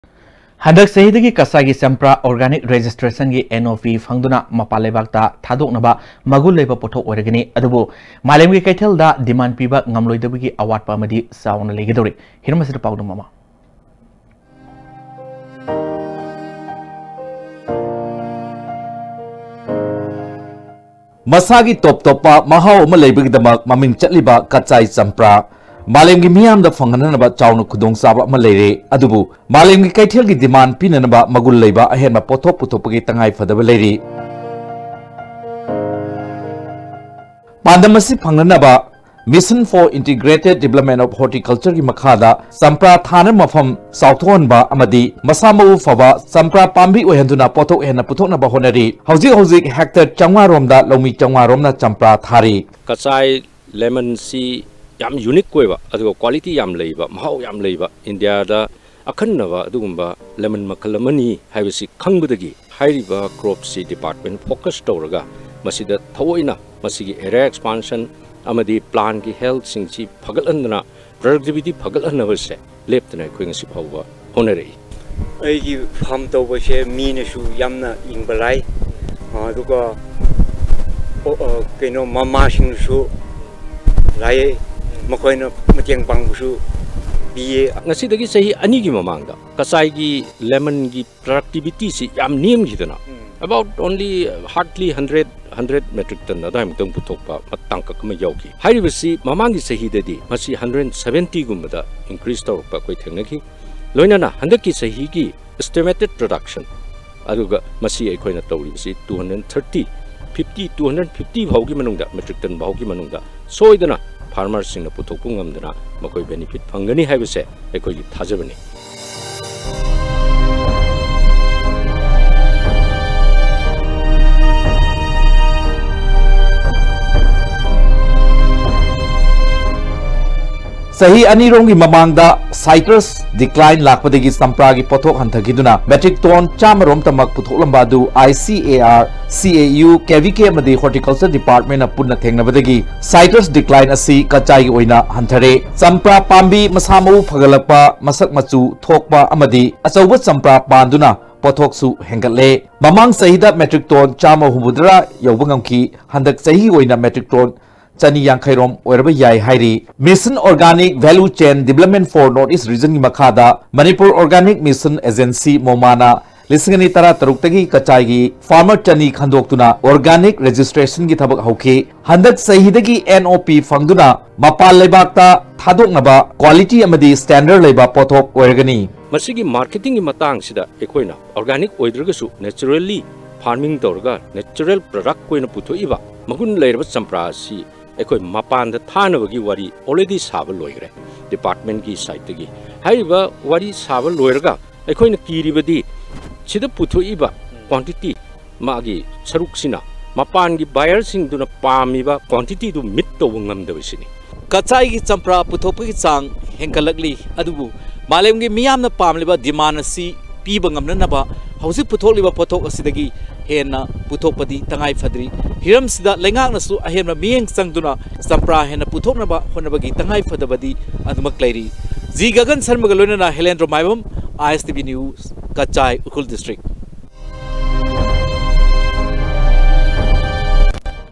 Kachaigi champra Organic Registrationgi NOP phangduna mapal leibakta thadoknaba potthok oirani hairi
Handak chahidagi Kachaigi champra Organic Registrationgi NOP phangduna mapal leibakta thadoknaba magun leiba potthok oiragani.  Adubu malemgi keithelgi demand piba ngamloidabagi awatpa amadi chaona leigadouri Hiramsida paodam ama...